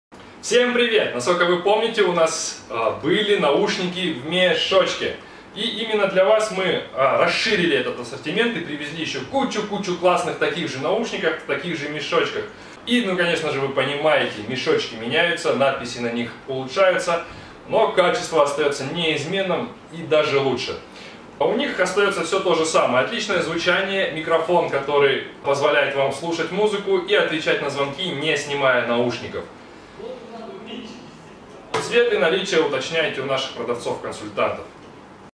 ОБЗОР! МИМИМИшные наушники в мешочке!
Всем привет!
Вашему вниманию наушники с чехлом в виде мешочка)))
В этот раз наш ведущий показывает как смешиваются слаймы и какие цвета получаются в итоге.
Если нравятся ролики с обзорами наших наушников, то ставь LIKE и каждая поддержка бесценна!))

По всем вопросам пишите на ватсап:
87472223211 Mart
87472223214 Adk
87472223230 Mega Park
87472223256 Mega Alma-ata